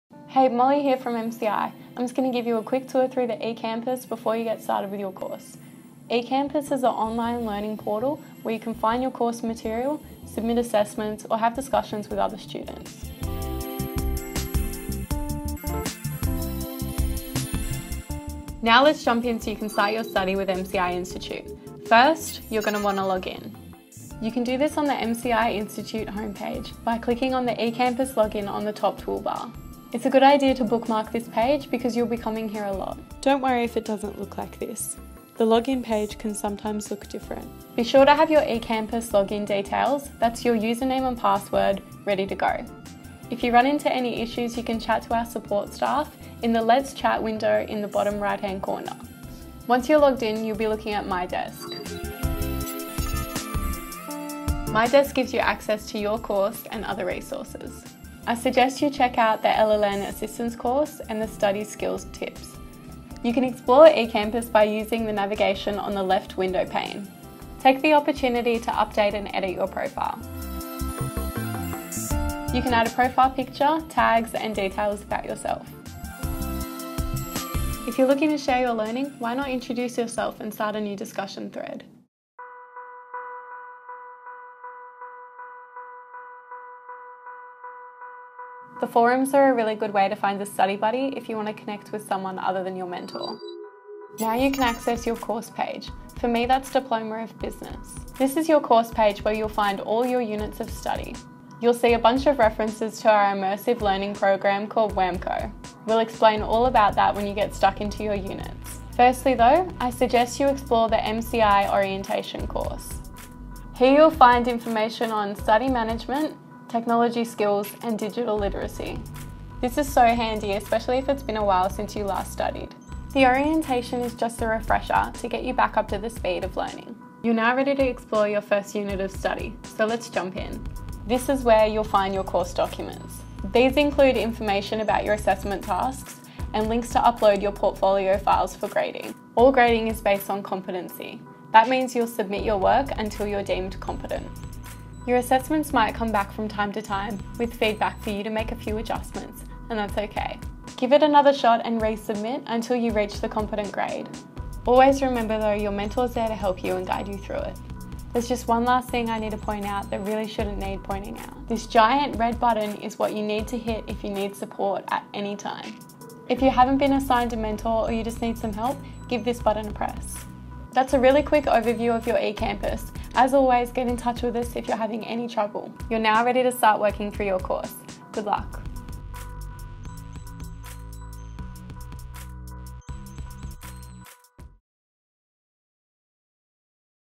A look around eCampus
Molly from MCI Institute takes you through the ins and outs of your eCampus.